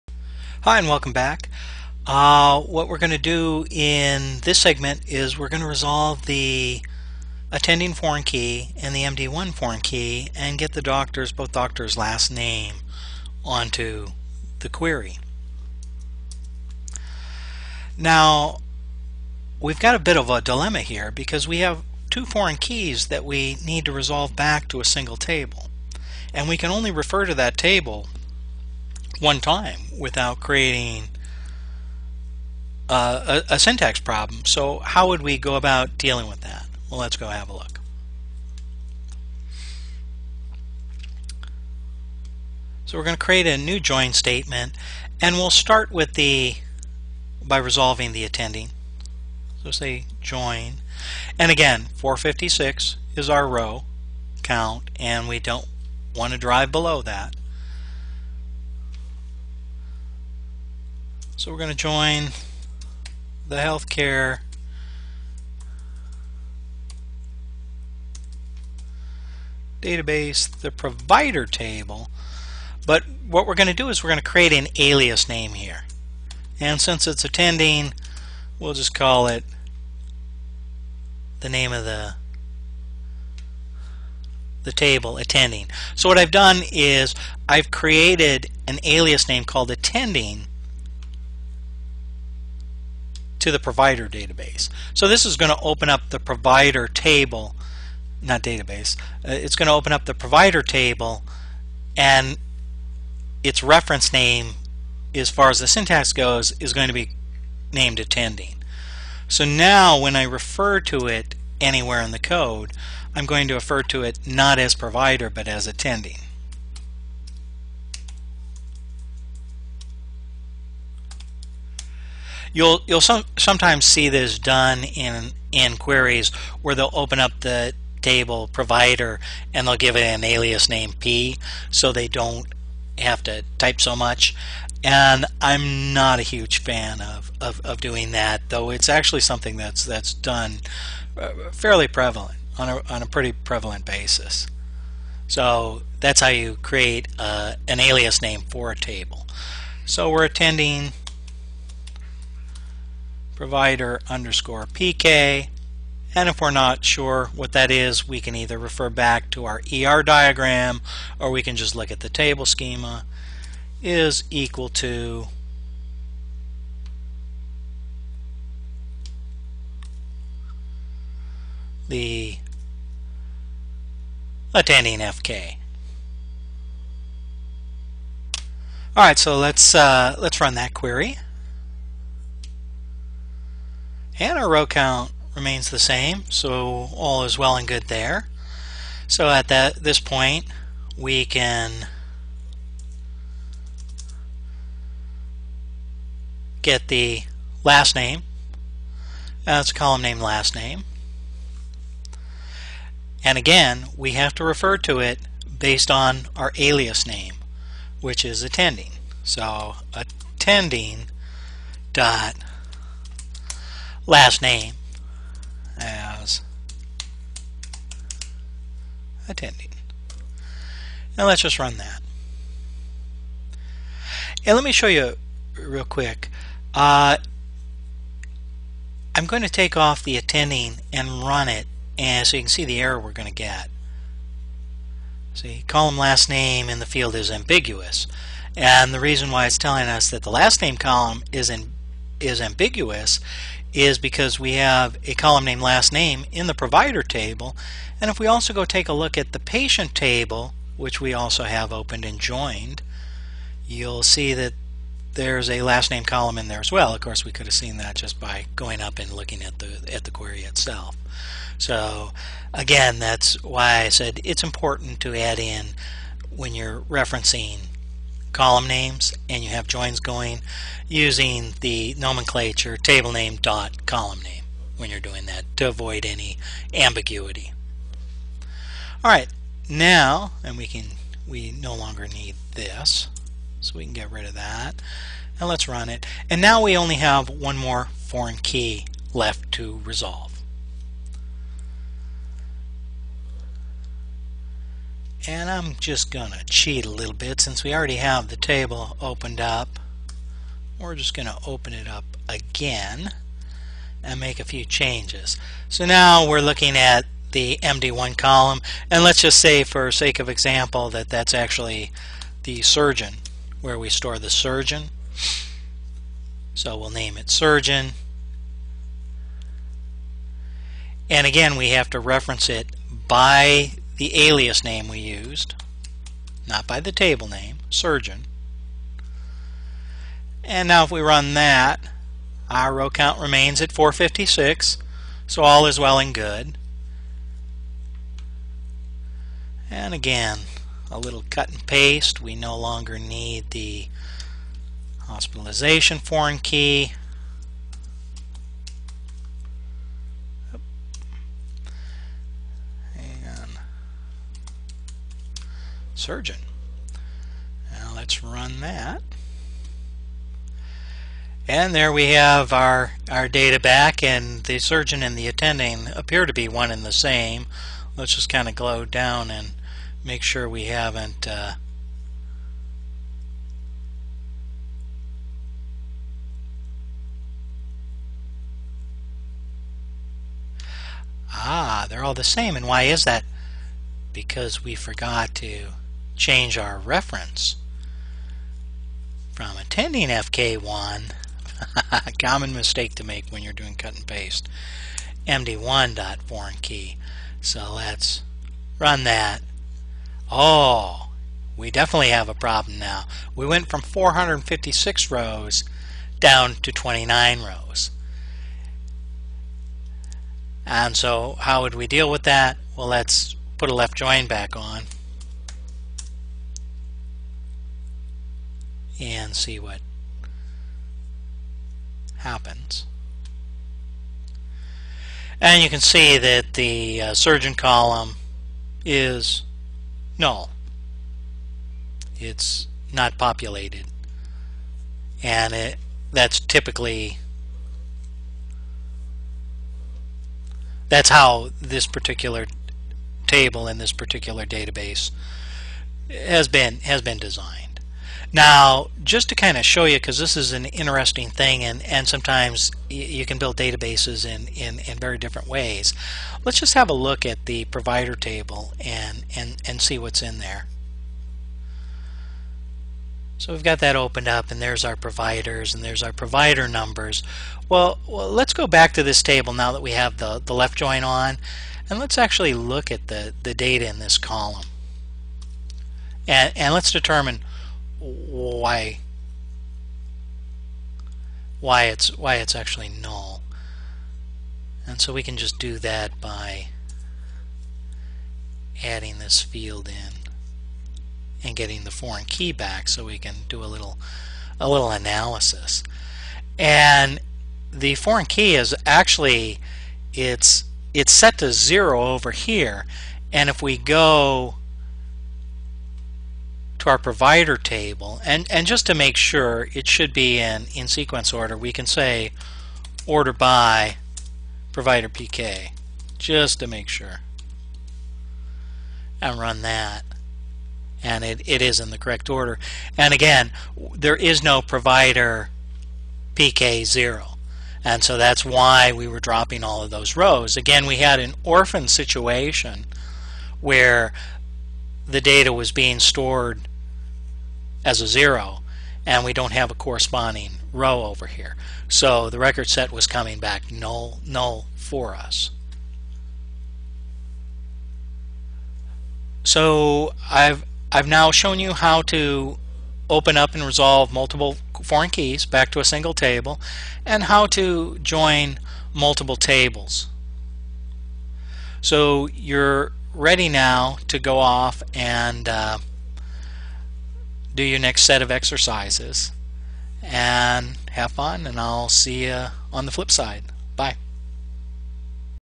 MySQL Querying Data - Lesson 7, Part 5 - Joining Tables - Multiple Fks to 1 Table
In this lesson we learn how to resolve multiple foreign keys back to a single table.